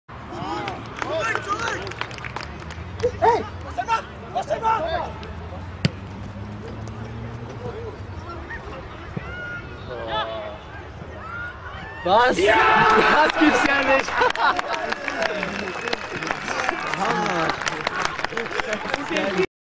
Thủ môn ghi bàn từ cú phát bóng lên
(Bongda24h) - Một khi đã được thần May mắn phù hộ thì các cầu thủ có thể lập công từ mọi vị trí, mọi khoảng cách. Đơn cử như anh chàng thủ môn trẻ tuổi trong đoạn clip dưới đây. Tận dụng cơ hội người đồng nghiệp trấn giữ khung thành của đối phương lao lên tham gia một tình huống tấn công và chưa kịp lùi về, thủ môn này lập tức phát mạnh về phía đối diện và thật bất ngờ, trái bóng đã chui vào lưới. Rõ ràng ở một cự ly xa như thế (ước tính trên 90m) mà không gặp "vận đỏ" thì làm sao có thể thành công.